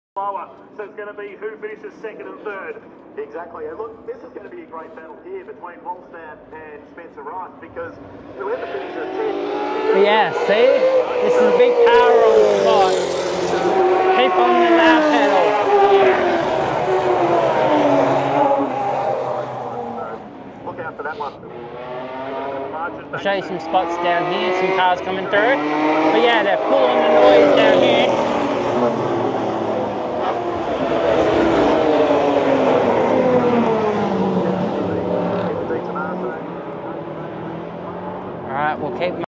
Group S Racing as seen at Bathurst - crowd footage
Great footage from stop Mt Panorama Bathurst. Great to see Group S cars running there once again since 2020.

Shame about the contact between the two Alfa’s, which was one of only two minor car to car contacts over the weekend. Otherwise great racing. Be sure to check out the footage from Race 1 on our channel.